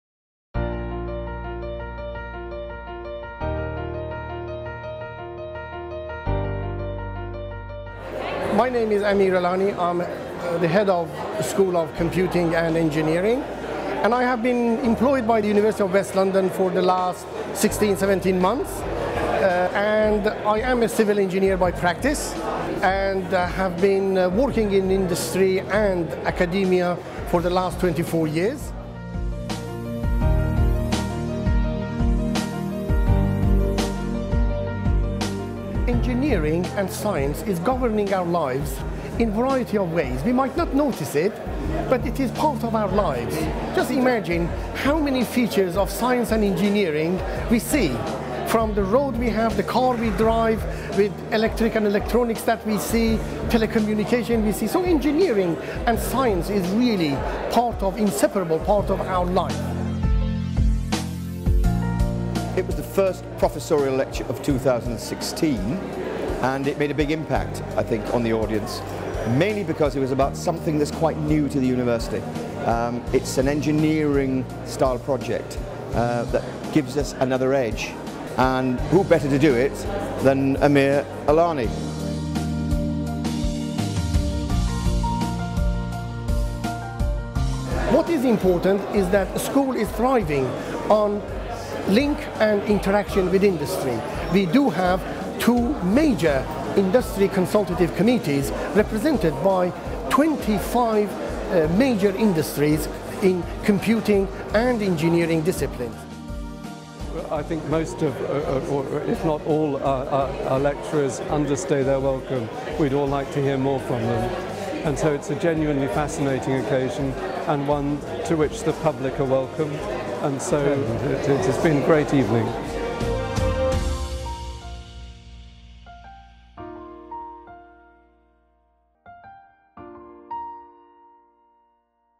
Applications of Ground Penetrating Radar - UWL Public Lecture
Professor Amir Alani, Head of the School of Computing and Engineering at the University of West London, discussed the varied uses of Ground Penetrating Radar at a UWL Lecture.

Professor Alani explained how the technology is used to assess, forecast and predict damage in various structures from the very old, e.g. the tomb of Tholos, Mycenaen Tomb of Archarnae, north east of Athens, to relatively modern structures such as highway bridges.

Using real case studies from his own work, Professor Alani, discussed the example of the Pentagon Road Bridge in Kent, where he was able to collect data revealing the effect of sustained heavy traffic on the structure of the bridge, and use this data to present solutions.